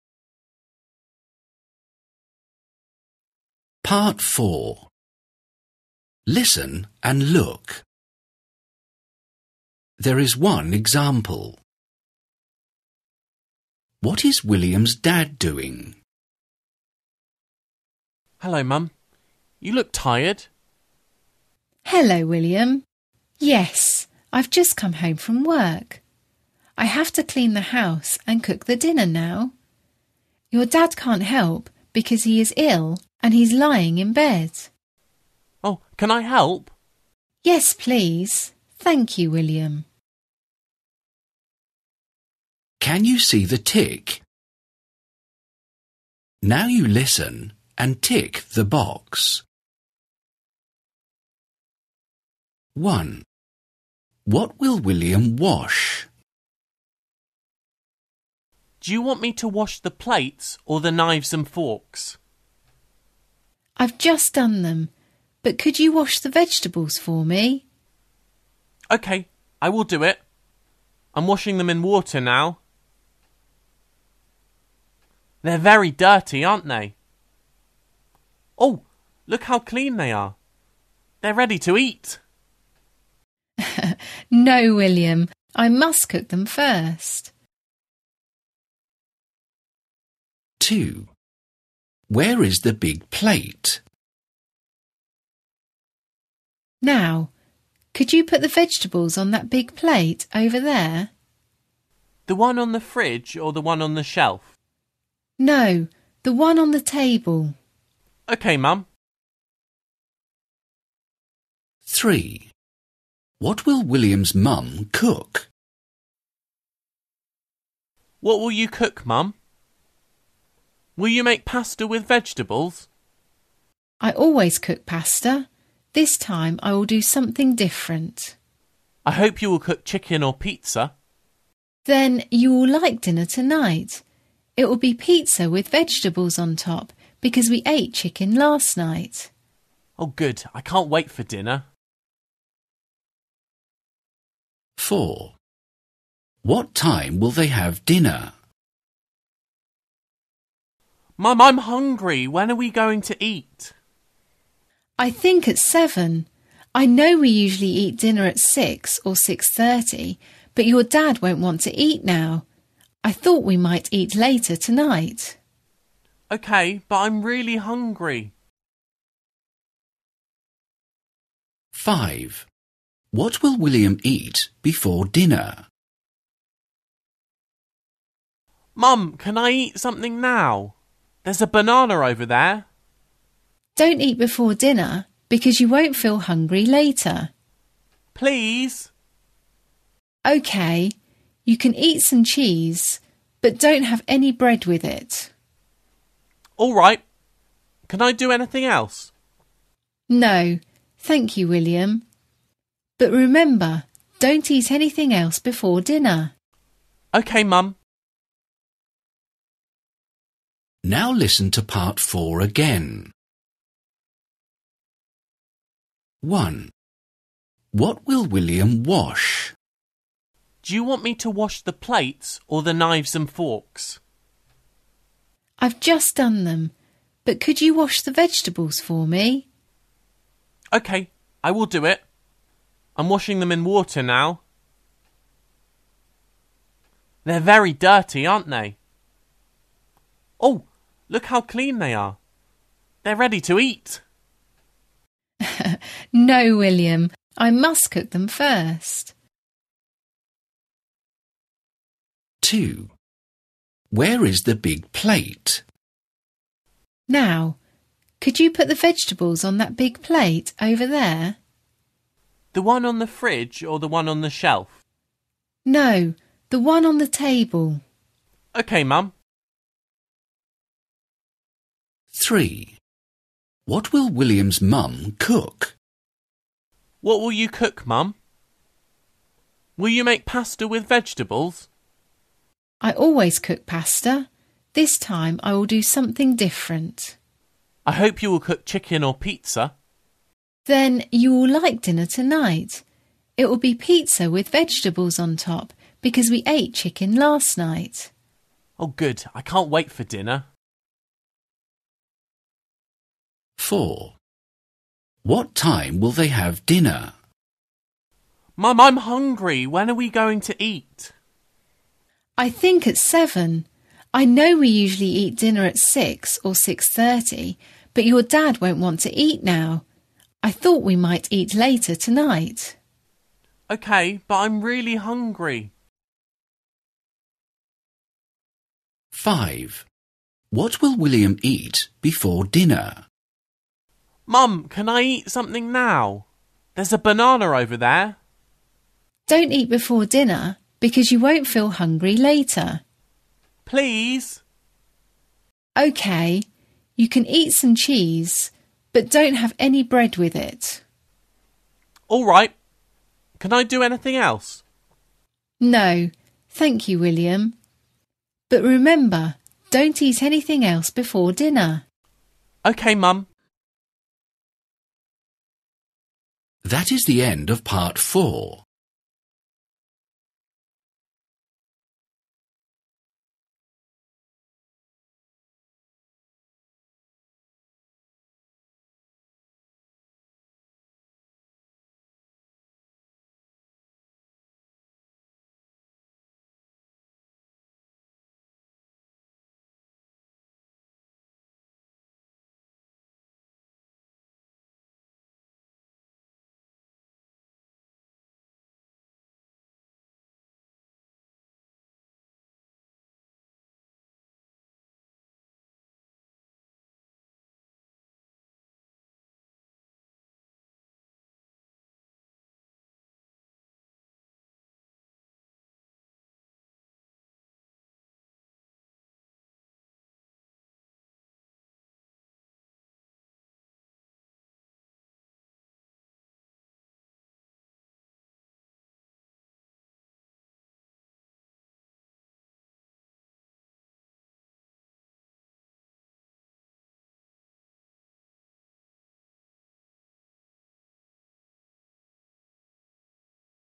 [2018] Succeed In Flyers Listening Test 7 Part 4
Succeed In Cambridge English flyers Listening Test 7 Part 4 (Revised exam from 2018)
Good luck!